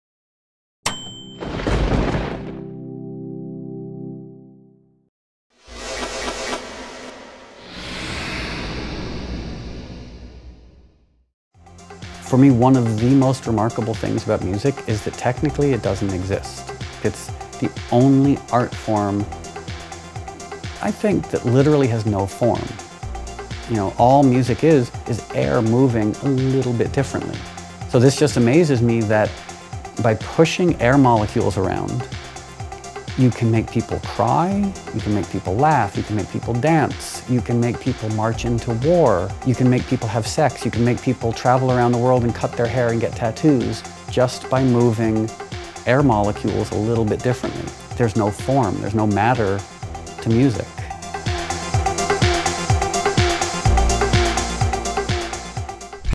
Mondays With Moby - Ep 3 - Does Music Exist?
In Episode 3, Moby reveals an interesting take on whether or not music actually exists.  As an artist who has sold millions of records, his answer will surprise you.